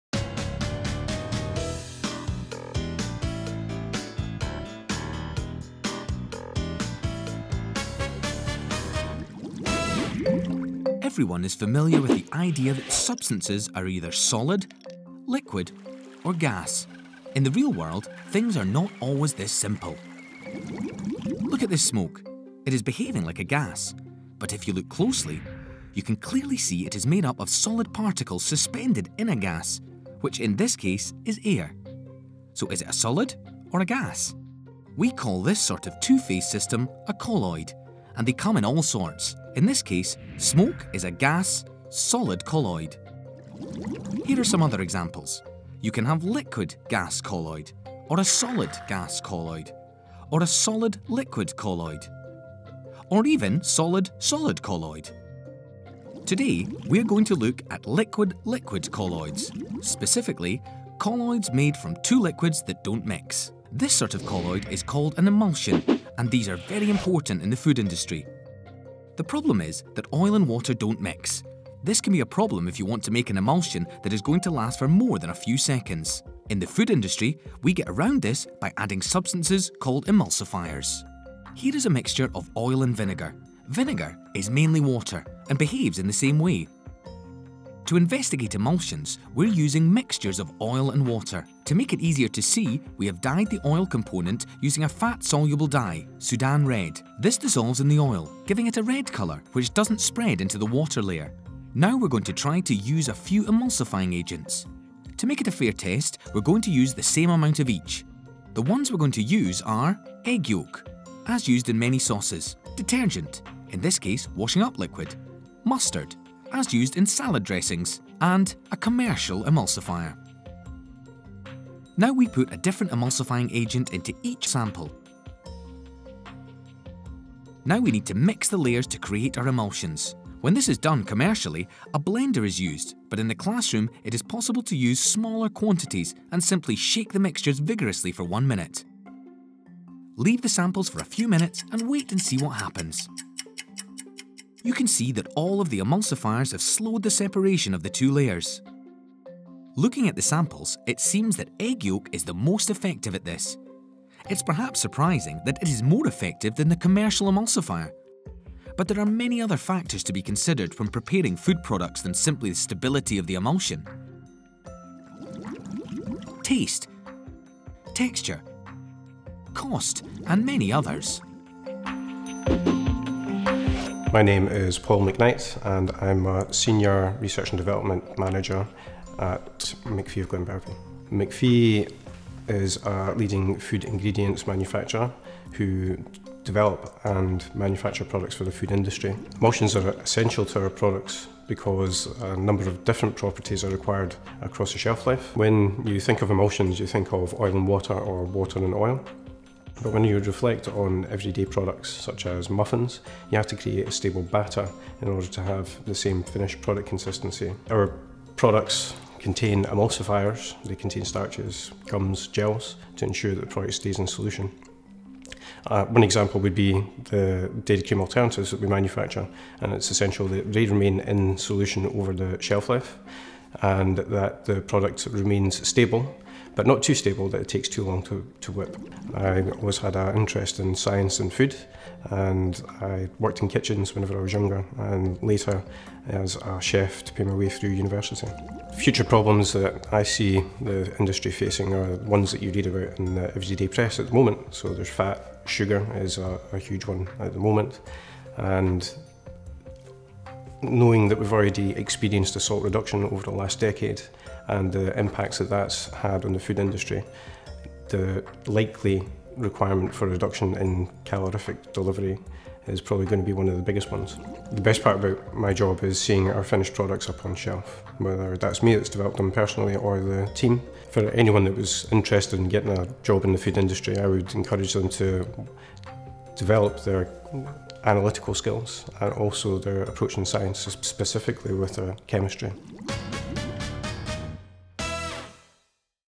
Emulsions
Emulsions are very important in the food and drink industry. This film provides several examples of food and drink related emulsions and also clearly demonstrates the use of liquid/liquid colloids and how different emulsifiers can affect their stability. This enables learners to understand the uses of this technique to the food and drink industry and provides a context for them to understand the relevance of emulsions.